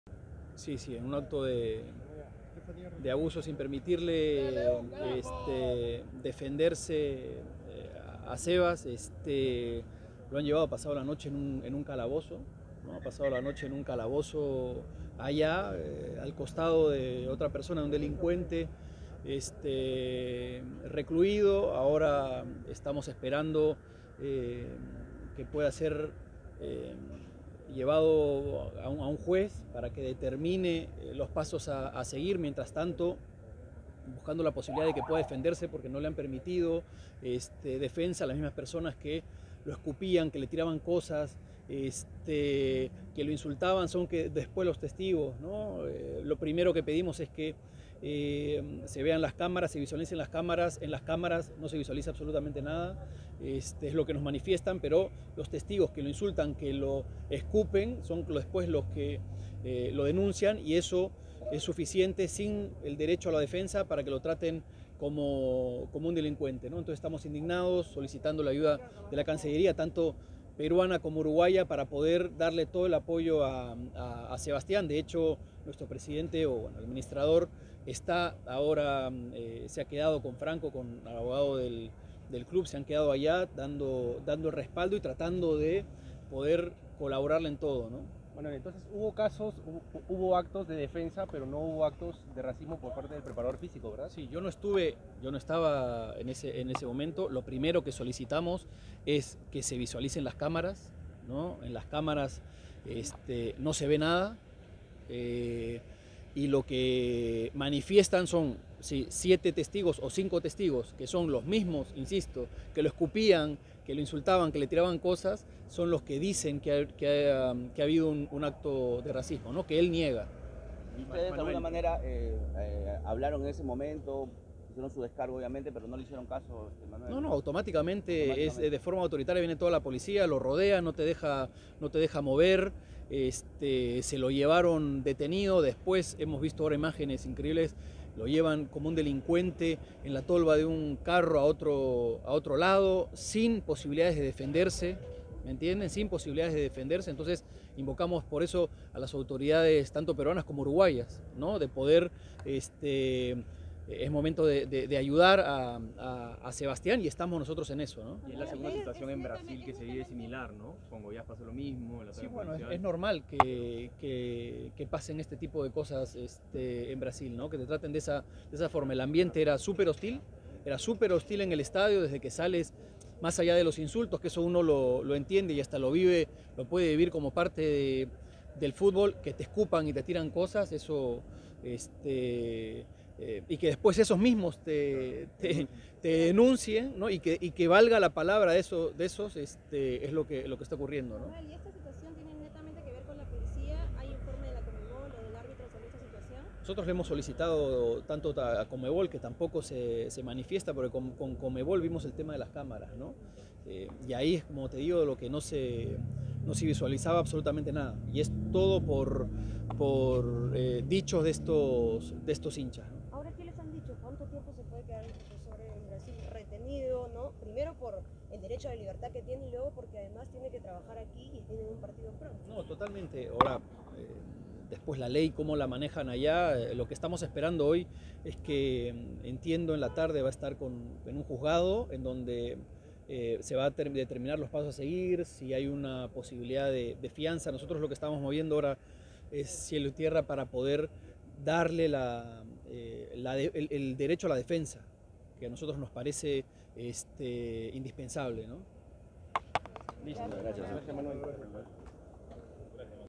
Manuel Barreto: "Estamos indignados, a Sebas (Avellino) lo han tratado como un delincuente"
El gerente deportivo crema se refirió a la detención del preparador físico de Universitario de Deportes en Sao Paulo.